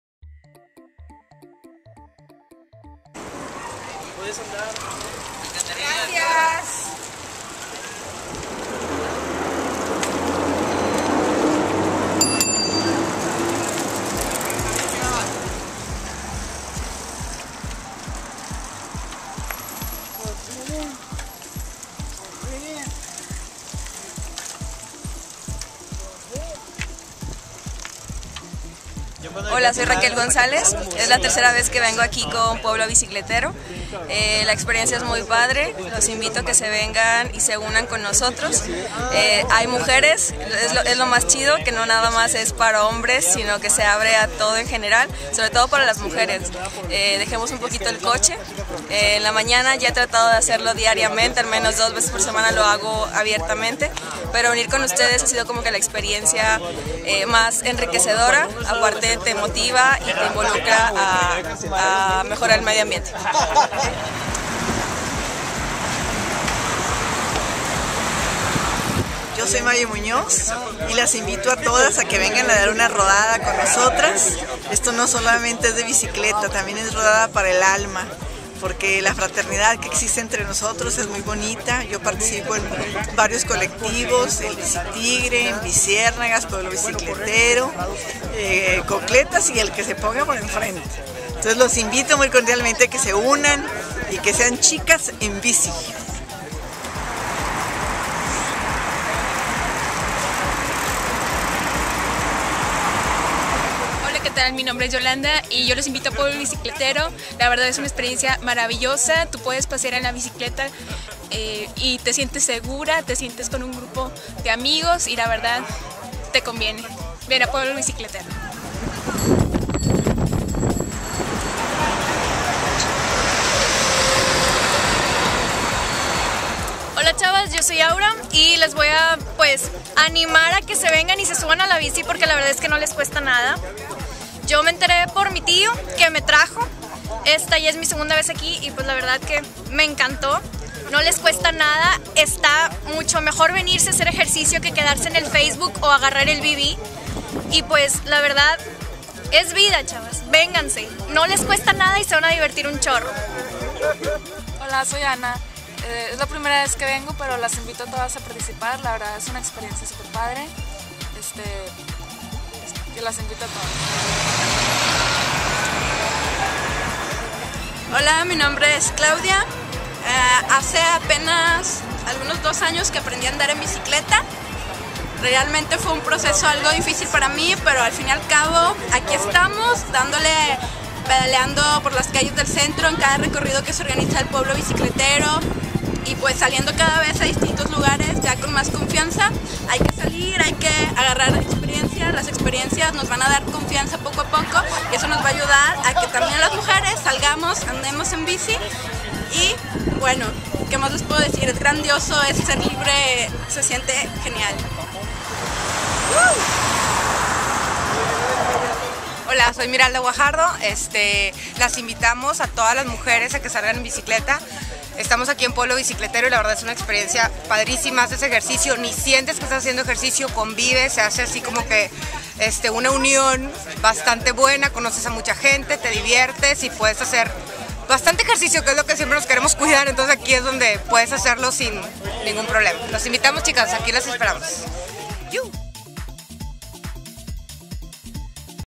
Bicicleteras invitan a todas las mujeres a subirse a la bici | Pueblo Bicicletero
En el marco del Día Internacional de La Mujer (8 de Marzo) mujeres ciclistas que han participado en los recorridos de Pueblo Bicicletero invitan a otras mujeres a subir a la bicicleta y te comparten sus razones. Qué esperas, anímate a subirte a la bici! En Bici Soy + Libre

Este domingo 11 de Marzo será la PEDALEADA EN FEMENINO. Todxs celebrando la participación de la mujer. Nos vemos a las 6pm en Colegio Civil. más información